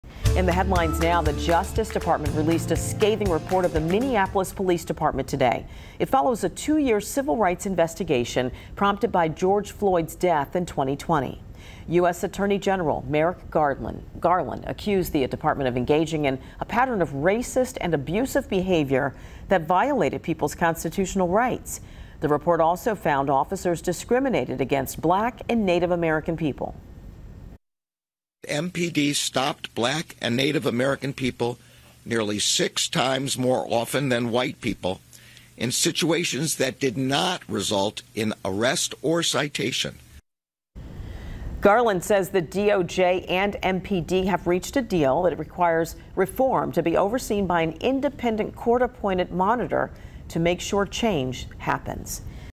DOJ probe after George Floyd's murder finds racist, discriminatory practices in Minneapolis PD
The probe was launched a day after former officer Derek Chauvin was convicted of murder and manslaughter for killing Floyd.